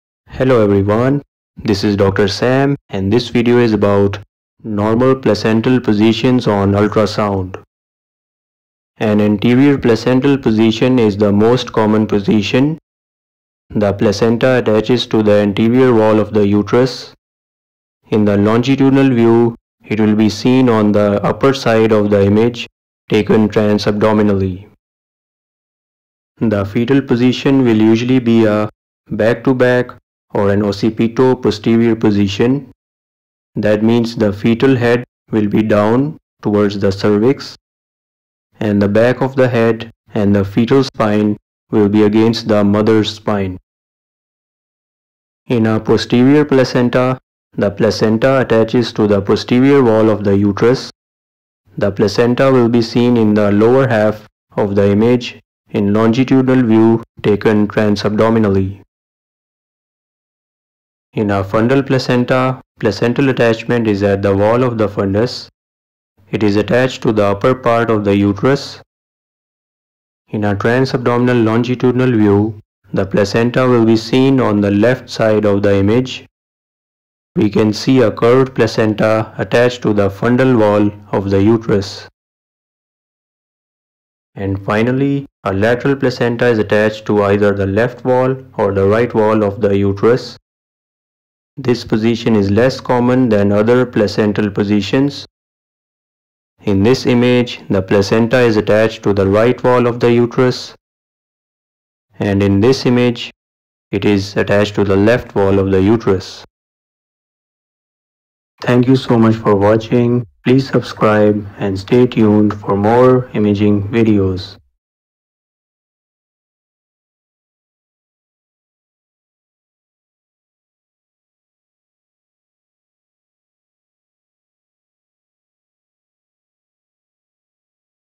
Placenta Positions On Ultrasound | Anterior/Posterior/Fundal/Lateral Placental Positioning USG
Cases Included:

Anterior Placenta
Posterior Placenta
Fundal Placenta
Lateral Placenta